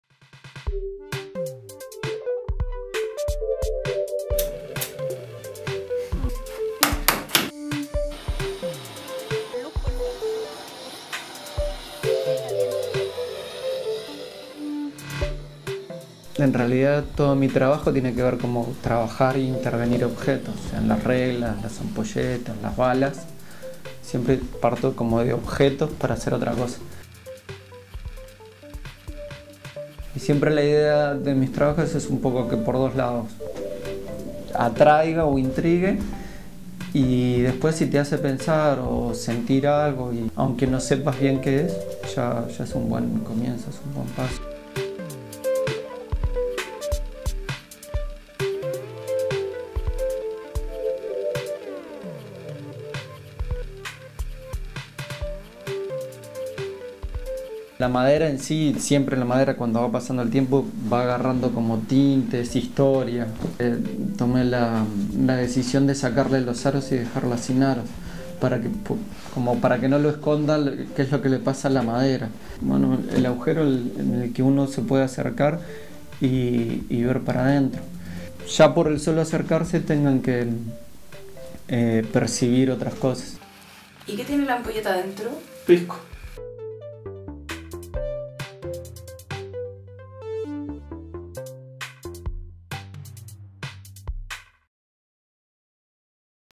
Vanguardia Mistral 2011: Pedro Tyler
Pisco Mistral reunió en 2011 a importantes exponentes del arte contemporáneo nacional en una original muestra de barricas intervenidas. Bajo la premisa "Cada Barrica es una Obra Maestra" se reunieron 12 artistas nacionales en "Vanguardia Mistral", elegidos tanto por su trayectoria como por la calidad de su trabajo. Esto es lo que hizo Pedro Tyler.